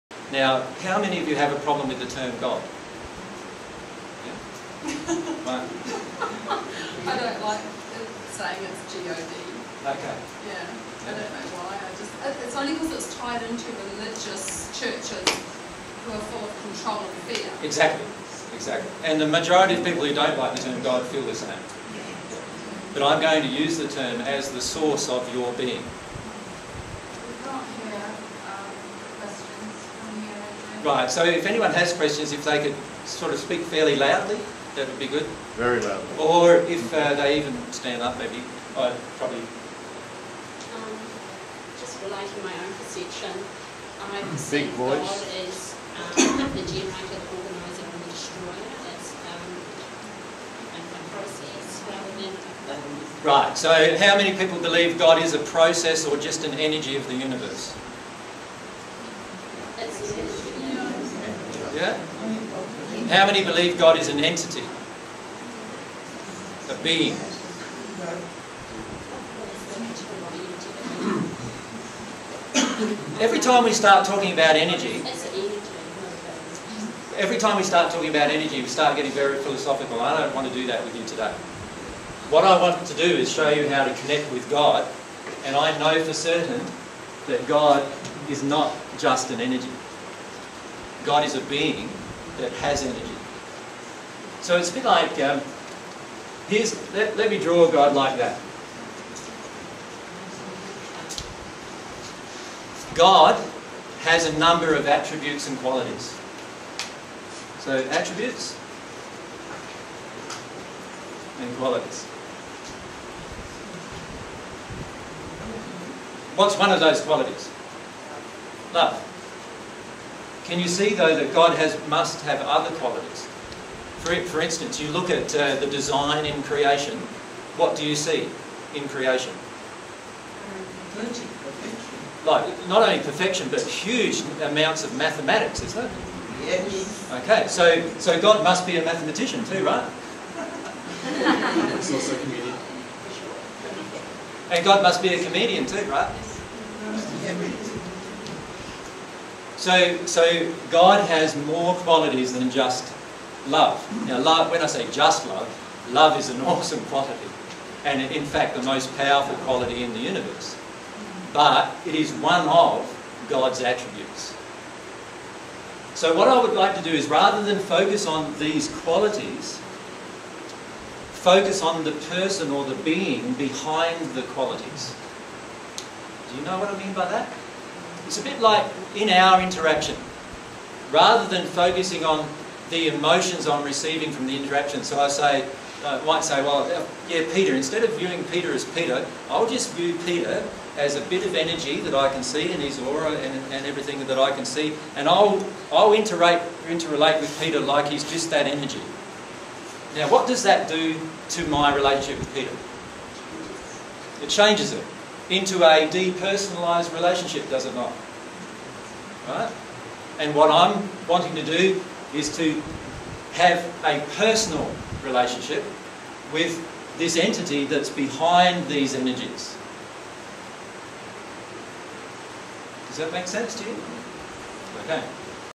Jesus on God Is More than Just Energy
Jesus explains that God is more than just an energy. He is a being with attributes and qualities with whom we can have a personal relationship.

This is a section of the presentation by Jesus entitled "20080120 Overview Of Divine Truth - Secrets Of The Universe".

dtstart:002623:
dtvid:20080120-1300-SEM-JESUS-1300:
dtmd5:427eb5154cb9b06b3e607fa778be0fb1: